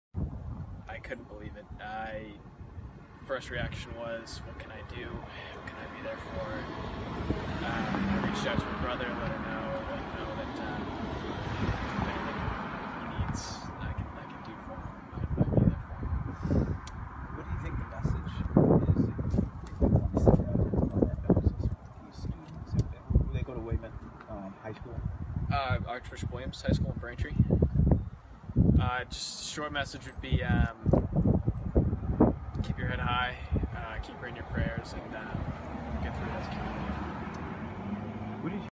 Cameron Dean goes to Archbishop Williams with some of the teens involved in the Weymouth crash. He v
Cameron Dean goes to Archbishop Williams with some of the teens involved in the Weymouth crash. He visited the site

By: Jessica Trufant
Published on: April 10, 2016